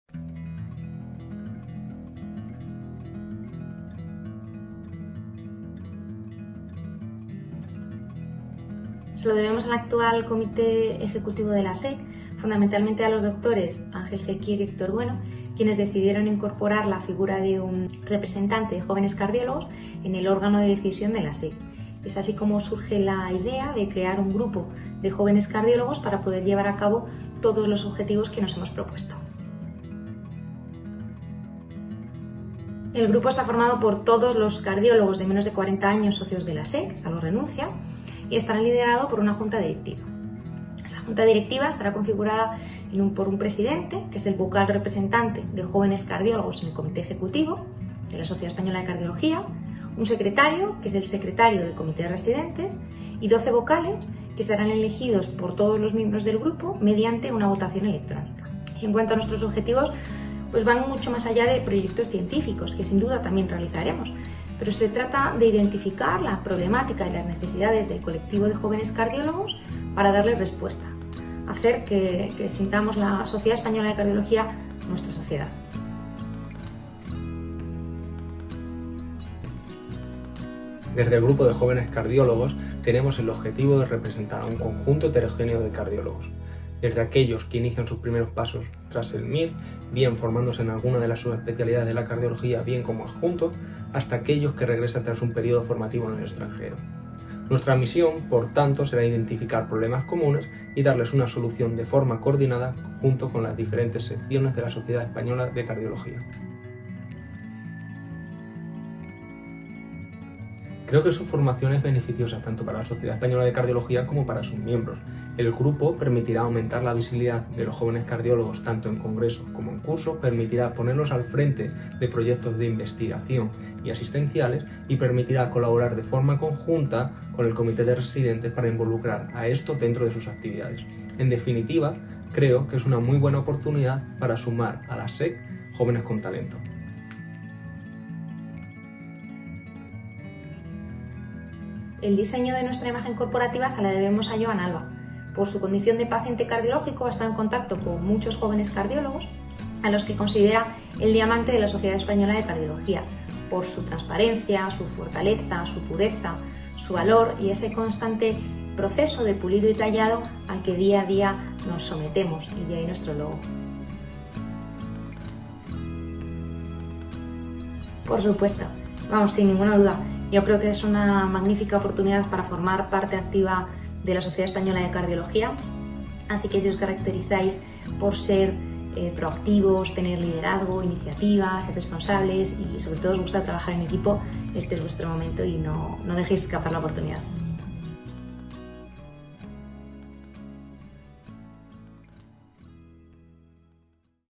Grupo Jóvenes Cardiólogos SEC
En el siguiente vídeo, los Dres. María Thiscal Lluva y Pedro Cepas, nos presentan el Grupo de Jóvenes Cardiólogos, nos cuentan cómo surgió la idea y qué retos y proyectos afronta.